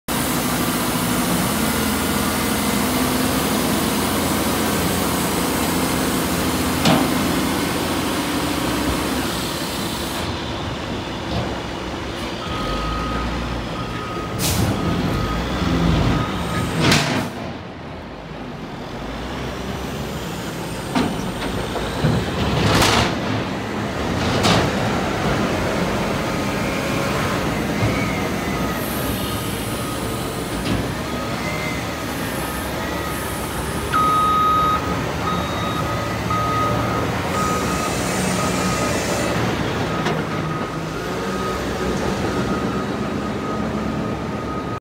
Dirt Day at RODEOHOUSTON
As RODEOHOUSTON gets ready for its 2020 show, NRG Stadium received hundreds of loads of dirt needed for the rodeo on Tuesday, February 25, 2020.